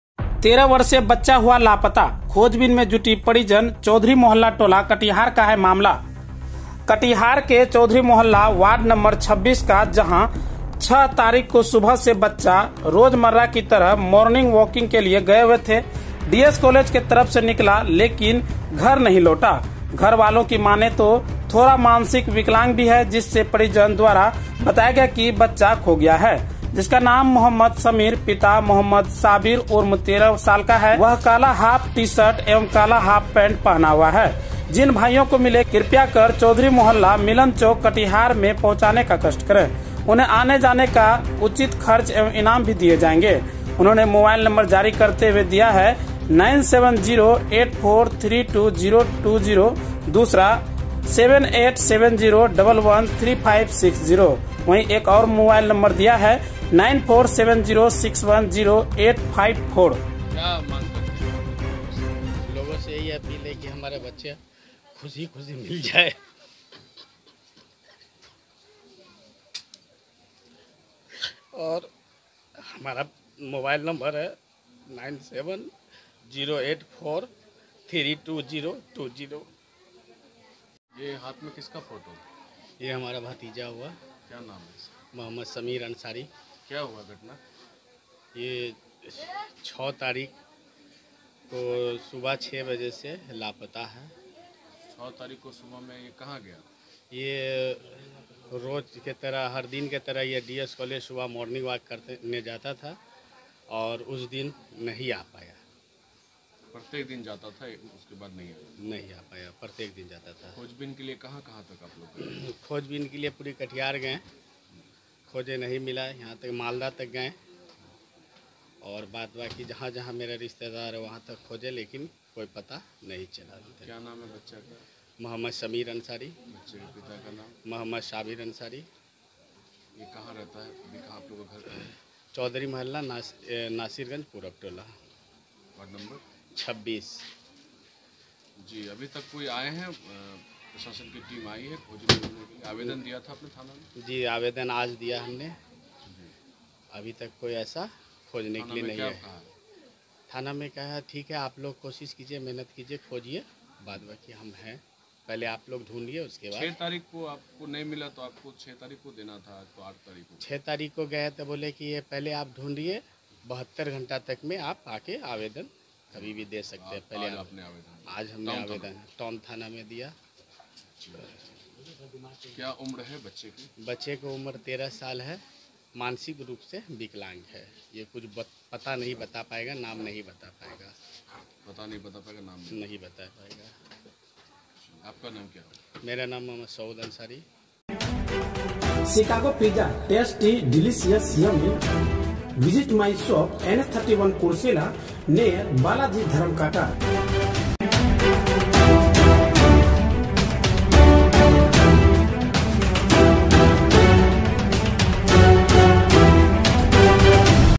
कटिहार : 13 वर्षीय बच्चा हुआ लापता। खोजबीन में जुटे परिजन।
मामला कटिहार के चौधरी मोहल्ला वार्ड नंबर 26 का जहां 6 तारिक को सुबह ये बच्चा रोजमर्रा की तरह मॉर्निंग वाकिंग करने घर से डीएस कॉलेज की तरफ निकला लेकिन घर नही लौटा, घर वालो की माने ये थोड़ा मानसिक विकलांग भी है जिसमें परिजन द्वारा बताया गया की बच्चा खो गया है जिसका नाम-मो० समीर, पिता-मो० साबीर उर्म: 13 साल का है वह काला हाफ टी-सर्ट एवं काला हाफ पैन्ट पहना हुआ है। जिन भाईयों को मिले कृप्या कर चौधरी मोहल्ला
मिलन चौक, कटिहार में पहुँचाने का कस्ट करें। उन्हें आने जाने का
उचित खर्च एवं इनाम दिया जायेगा ।
मोबाईल नं० पर संपर्क कर सकते है